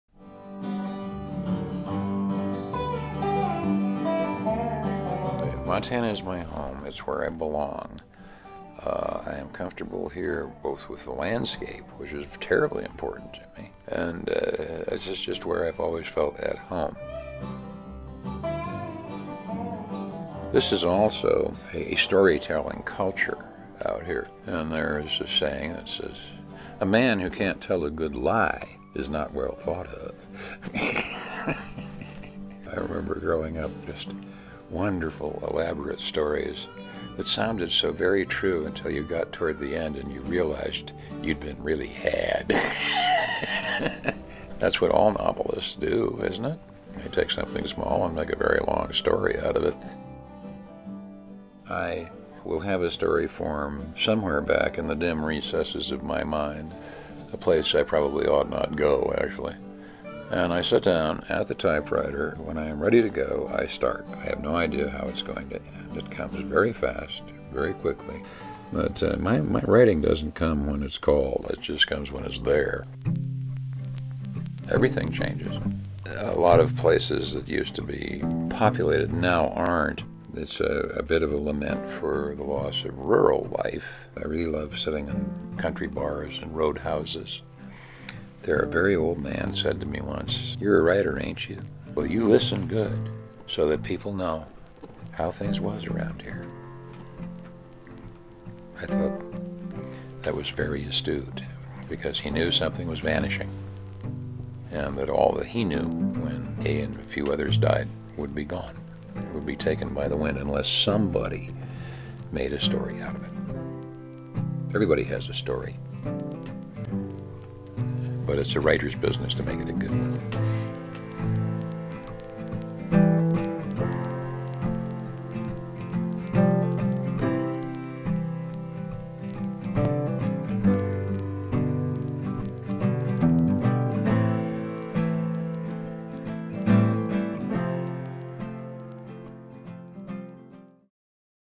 Meet Peter Bowen
"My writing doesn't come when it's called.  It just comes when it's there." shares Peter Bowen, author of The Montana Mystery series featuring Gabriel Du Pré—a mixed-race lawman living in fictional Toussaint, Montana.  "I'll have a story formed somewhere in the recesses of my mind, and I sit down at the typewriter.  When I'm ready to go, I start.  I have no idea how it's going to end and it comes very fast. I think everybody has a story—it's a writer's business to make it a good one."